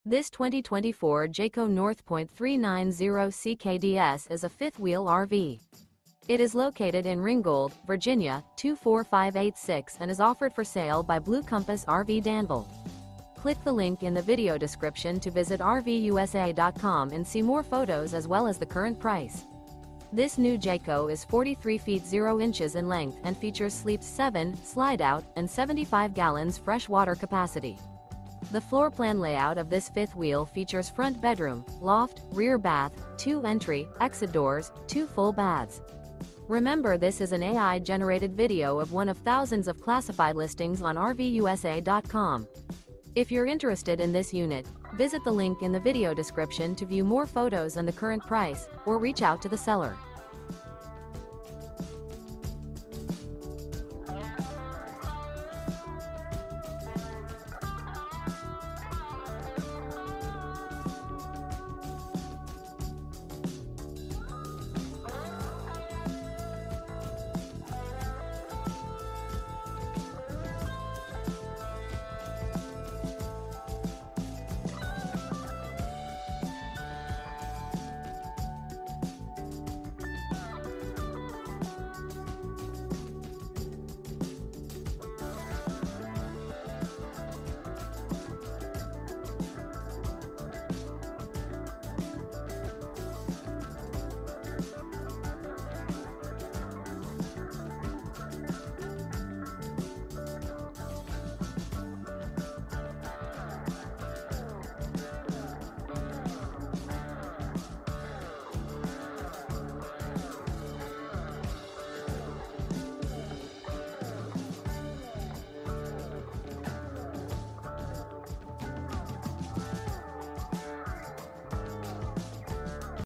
Wonderful 2024 Jayco North Point Fifth Wheel RV For Sale in Ringgold, VA | RVUSA.com
This 2024 Jayco North Point 390CKDS is a Fifth Wheel RV. It is located in Ringgold, VA 24586 and is offered for sale by Blue Compass RV Danville. This New Jayco Is 43' 0" in length and features sleeps 7, Slideout, and 75 gal fresh water capacity. The floorplan layout of this Fifth Wheel features Front Bedroom, Loft, Rear Bath, Two Entry/Exit Doors, Two Full Baths.